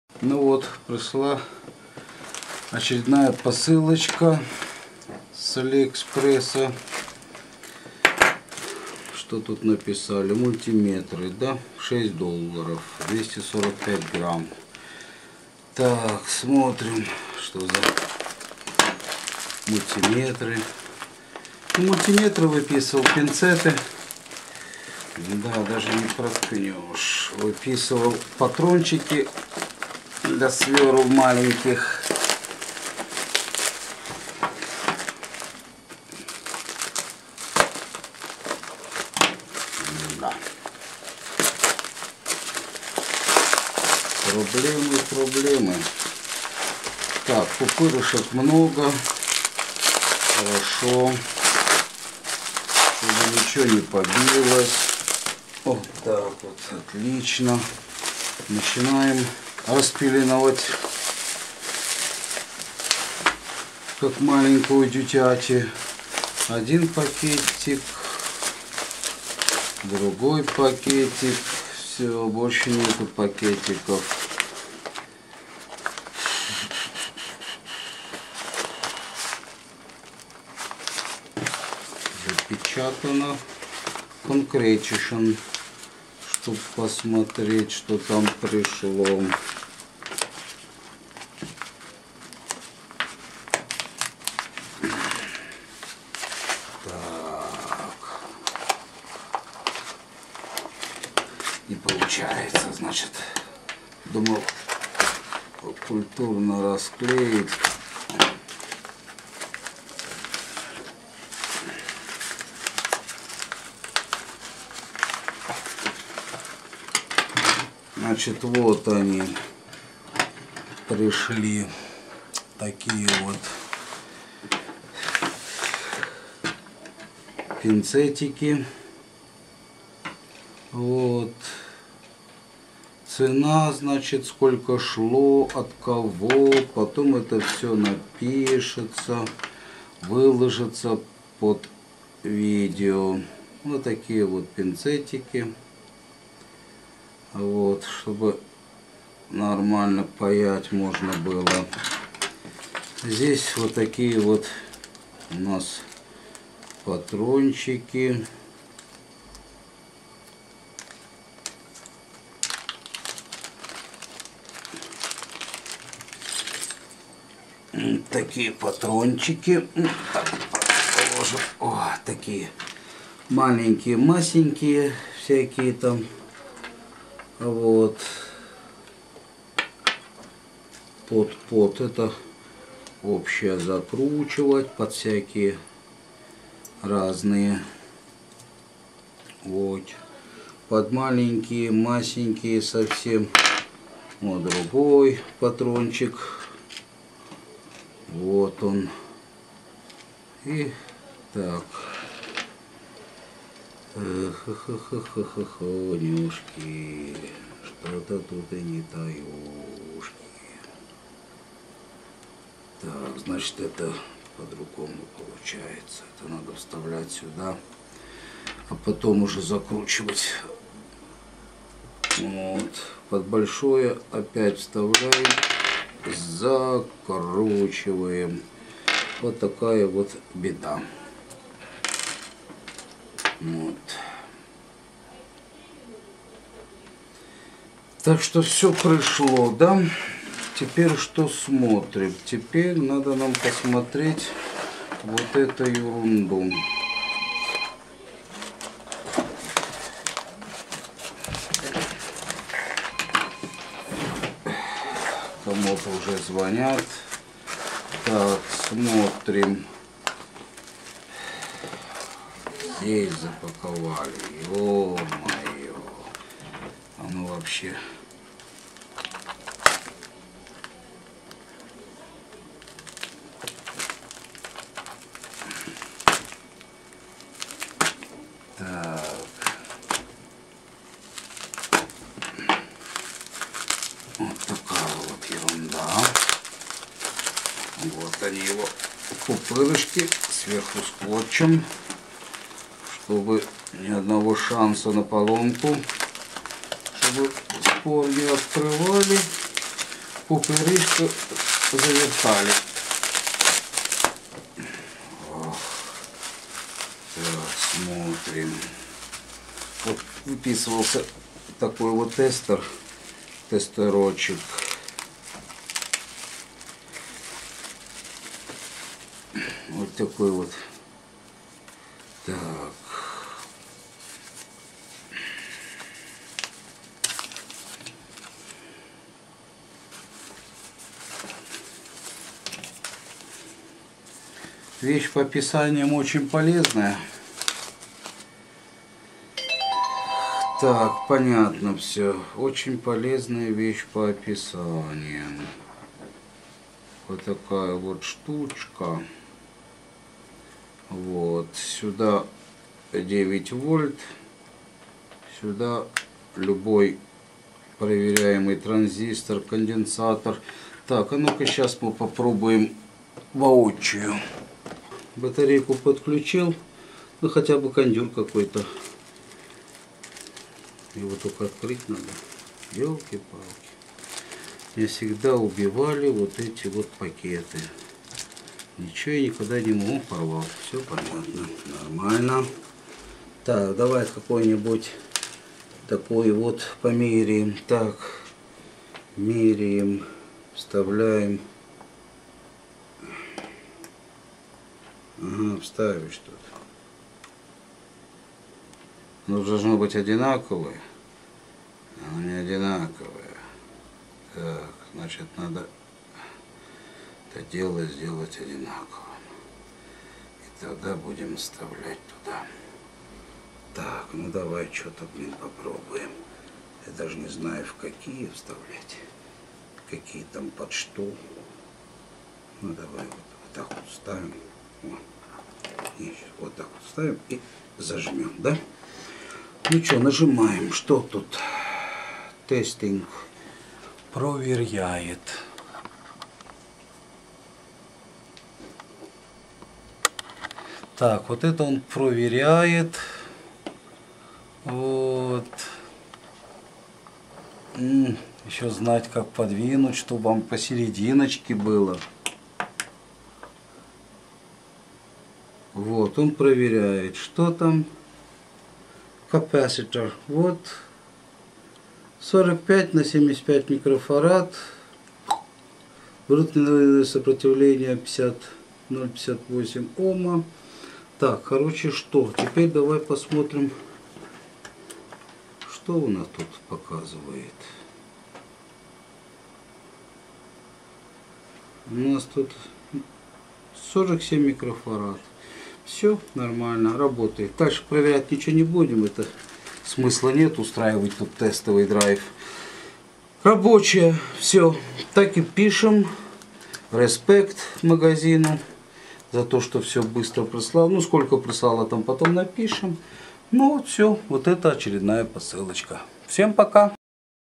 Пинцеты, патроны для маленьких свёрл и мультиметр с Алиэкспресс
Магазин ShenZhen Cycwiss Technology Co., Ltd с доставкой в 26 дней прислал пинцеты, патроны для маленьких свёрл и мультиметр. Все хорошего качества, а тестер понравился больше всего - меряет все и с полной информацией.